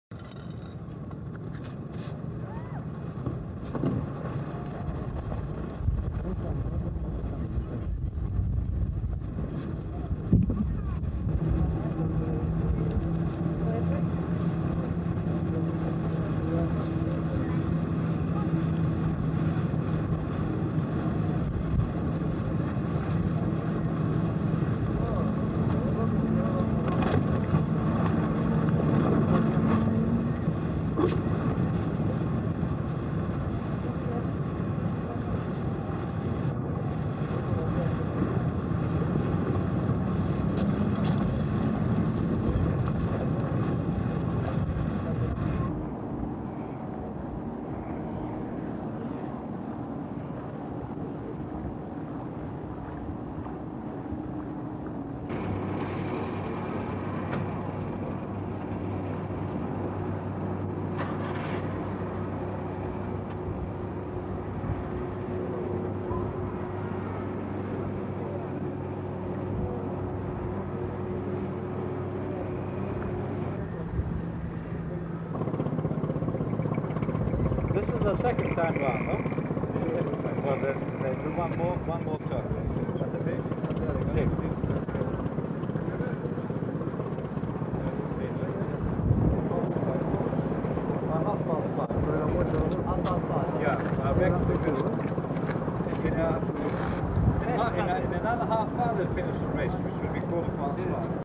RGYC Victory Class Sailing in the 1980s - 1
Victory class racing yachts of the Royal Gibraltar Yacht Club in action in the 1980s.

Featuring:

V6 Niña
V18 Boreas
V5 Cora
V17 Emendek (1)
V19 Alitea
and others.